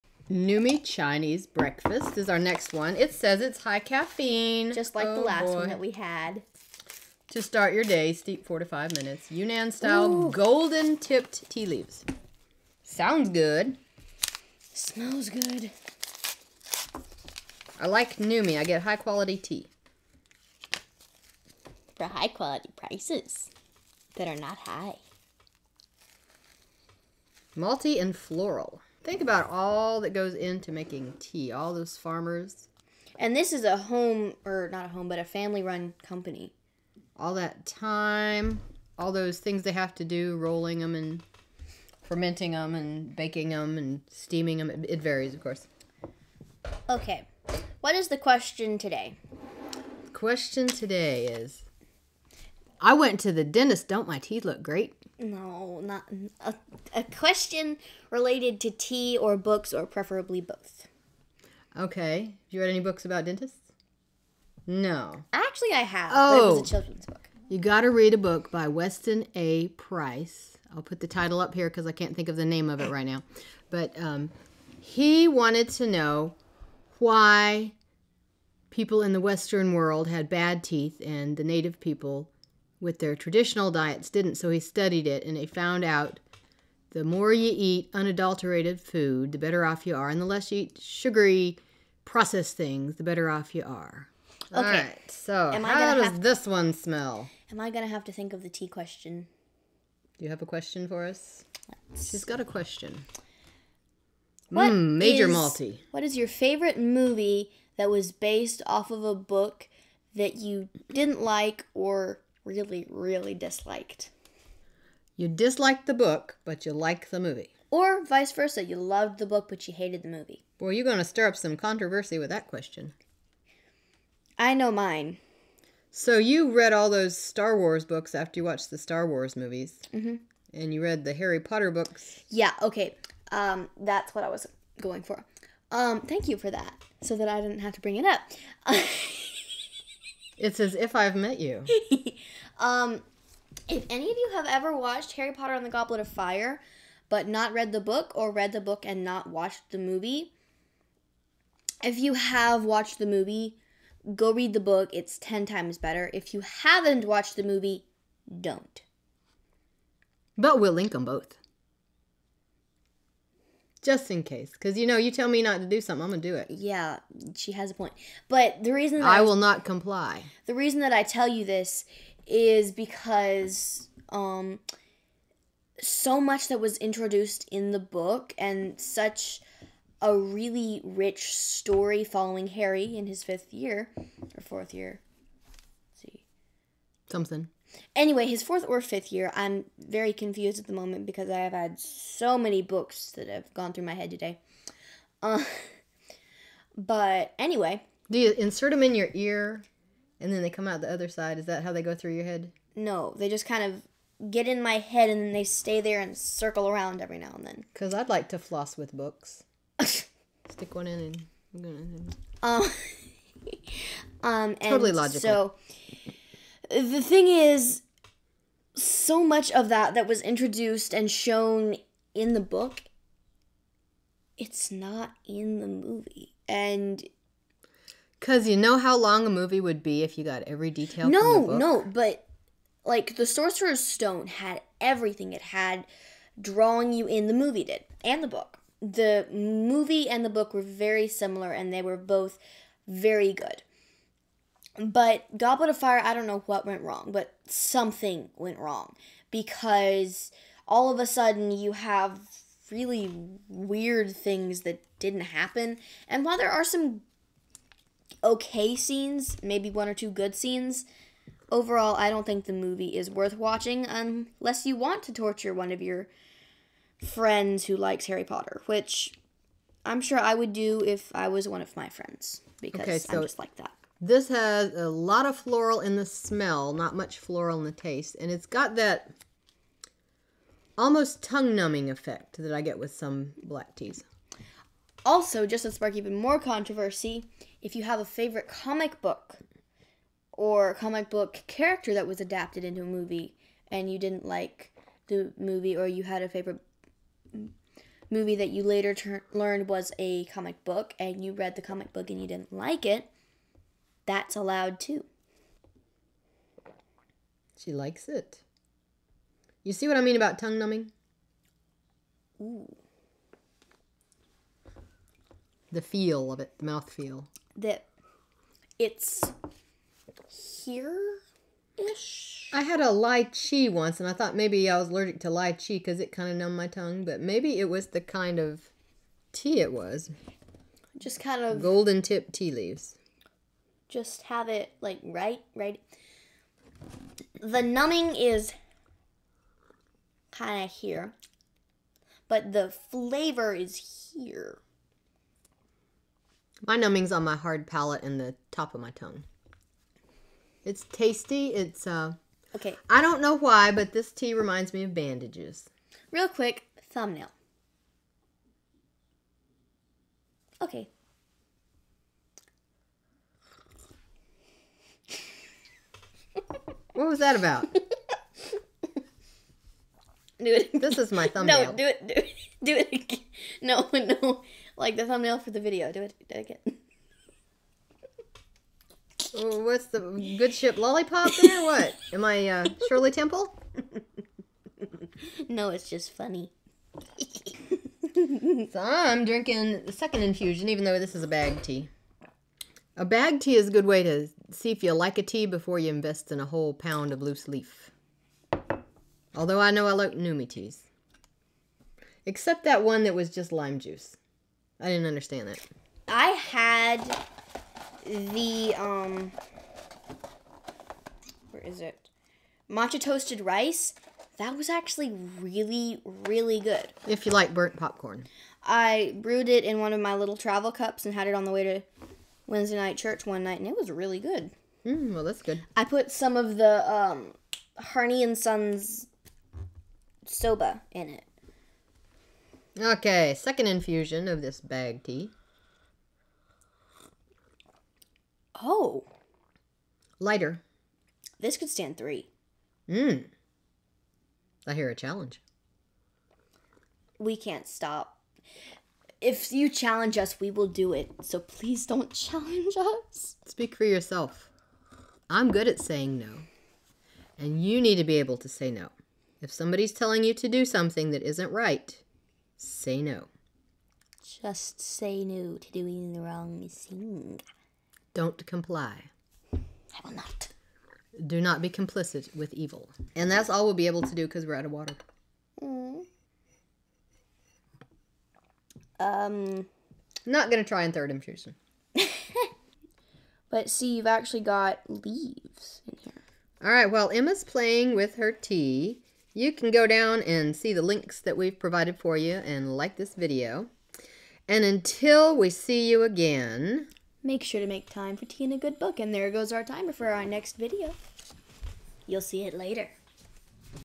We Try Numi Chinese Breakfast Tea
We like breakfast tea, and this one from Numi contains plenty of caffeine.

Numi Chocolate Rooibos
Numi Chinese Breakfast
Numi Breakfast Blend